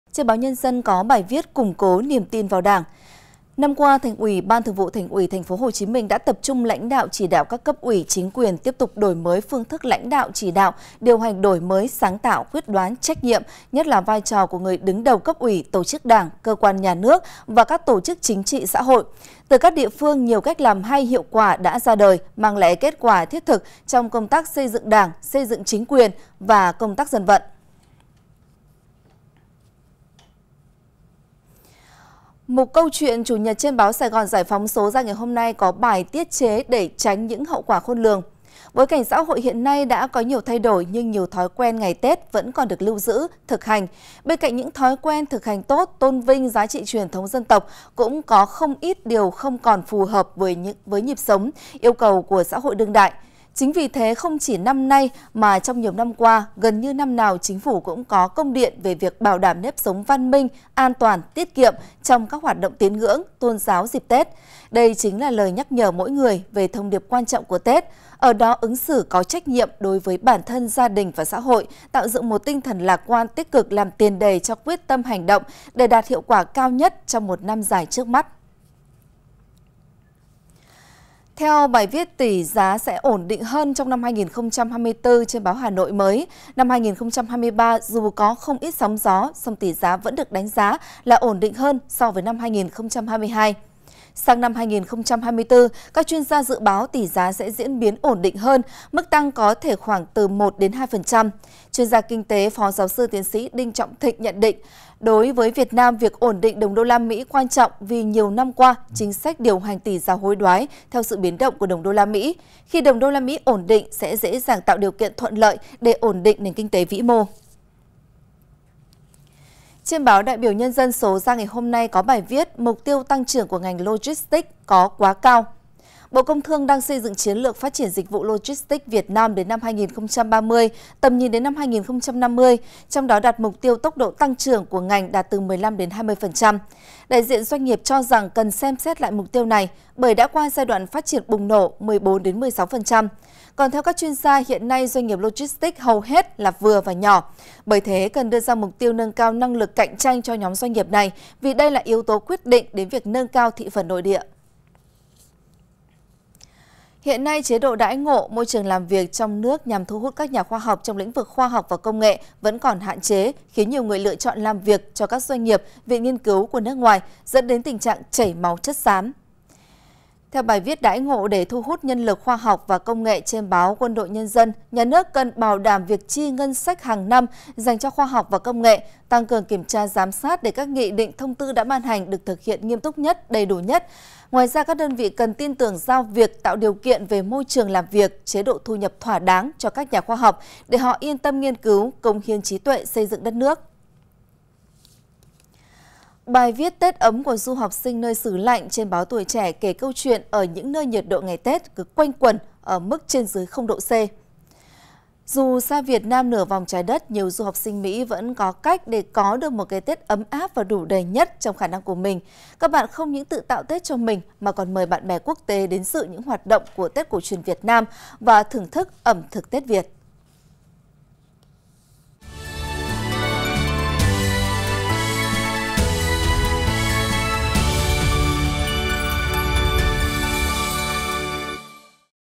Củng cố niềm tin vào Đảng | ĐIỂM BÁO | Đảng với Dân
Với quyết tâm cao của cả hệ thống chính trị, cùng sự đồng thuận của nhân dân, doanh nghiệp, công tác xây dựng Đảng, xây dựng chính quyền, công tác dân vận ở Thành phố Hồ Chí Minh năm 2023 đã đạt những kết quả tích cực, tạo nền tảng vững chắc cho thành phố thực hiện tốt nhiệm vụ chính trị, kinh tế-xã hội đã đề ra.